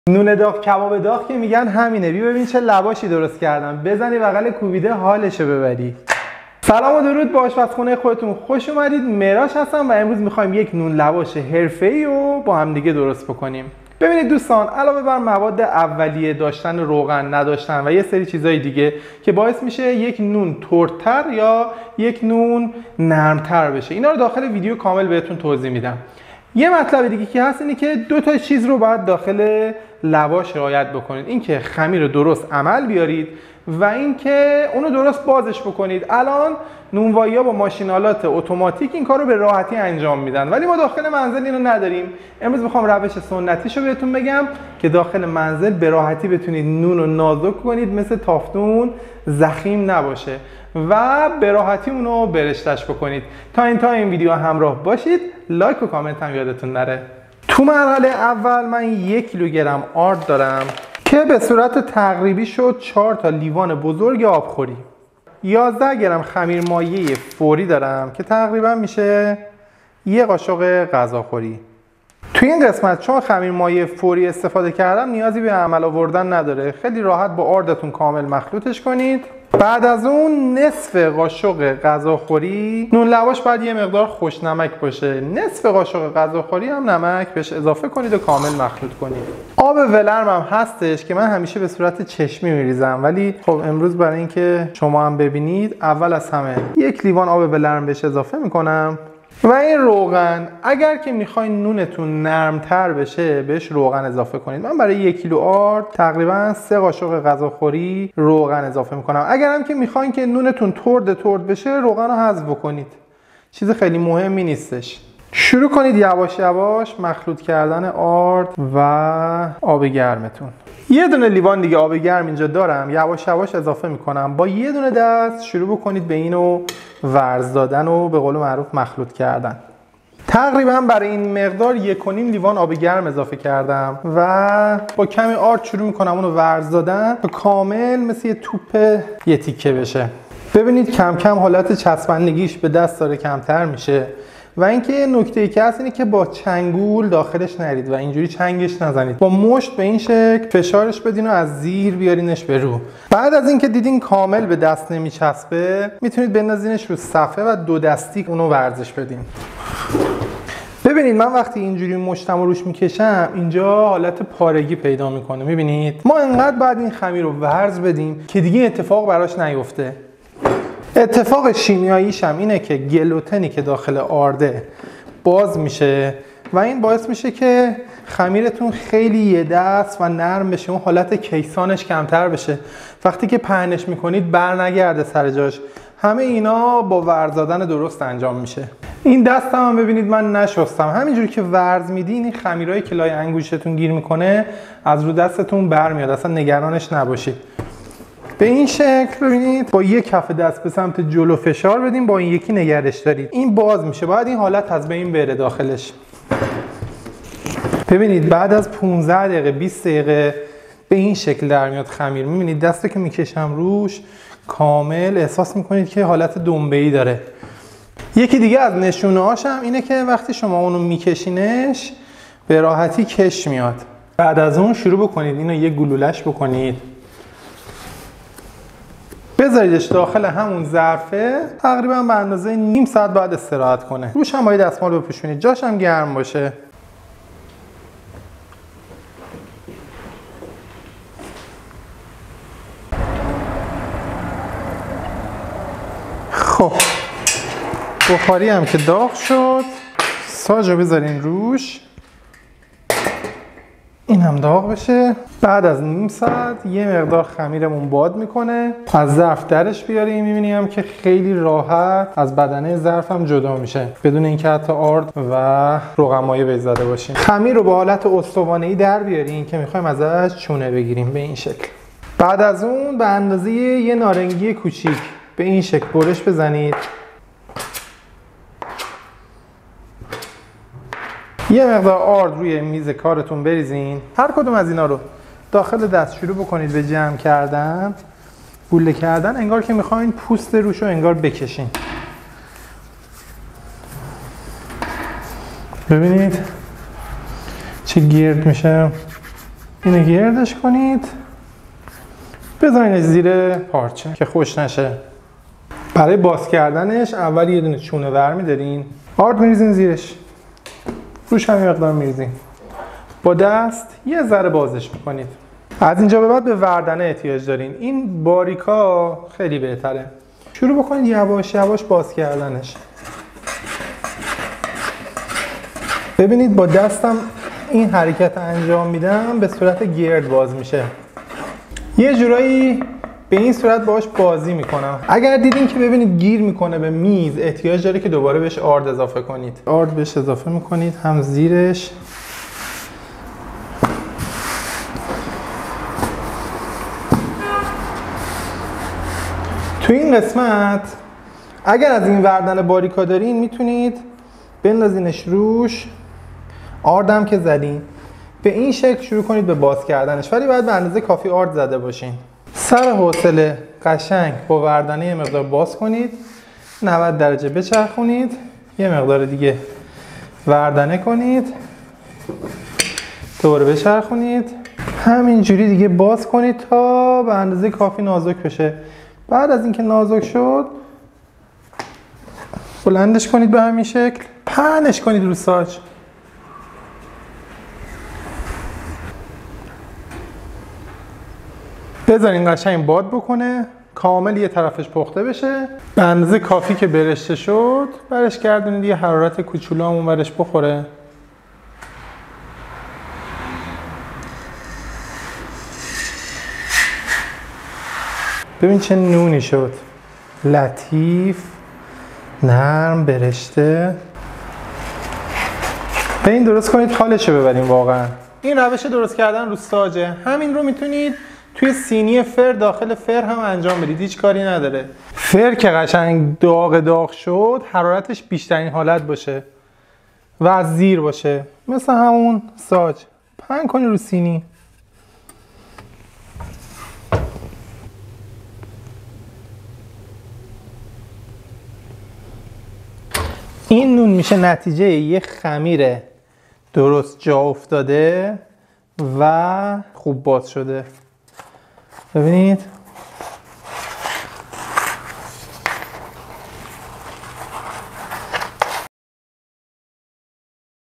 نان لواش حرفه ای و کامل ترین شکل خمیر گیری و پهن کردن خمیر
دوستان امروز داخل این ویدیو کامل ترین وآموزشی که می شد برای نان لواش انجام داد رو براتون گزاشتم از عمل آووردن خمیر تا باز کردنش به شکل حرفه ای جوری که قدیمی ها انجام میدادن و امیدوارم که لذت ببرید

مواد لازم
آرد سفید گندم: ۱ کیلو
خمیر مایه: ۱ قاشق غذاخوری یا ۱۱ گرم
نمک: نصف قاشق غذاخوری
آب ولرم: ۱ و ۱/۲ لیوان آبخوری
روغن: ۳ قاشق غذاخوری